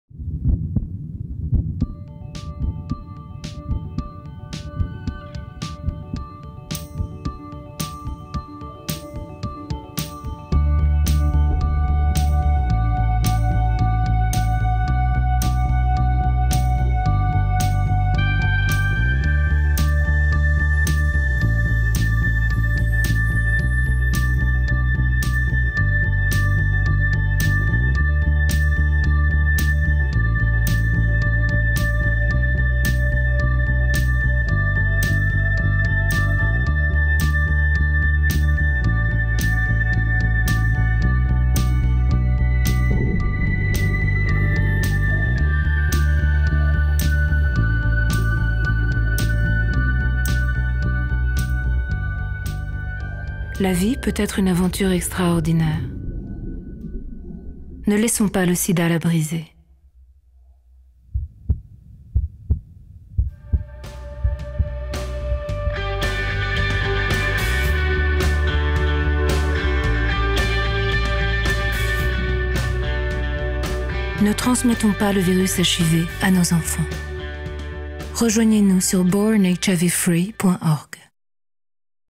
Born HIV Free : A l'intérieur - Ne laissez pas le Sida détruire leur avenir
Le risque d'infection par le VIH est une menace réelle pour les  fœtus portés par des mères vivant avec le virus.  Mais avec un traitement adapté, plus aucun enfant ne devrait naître séropositif.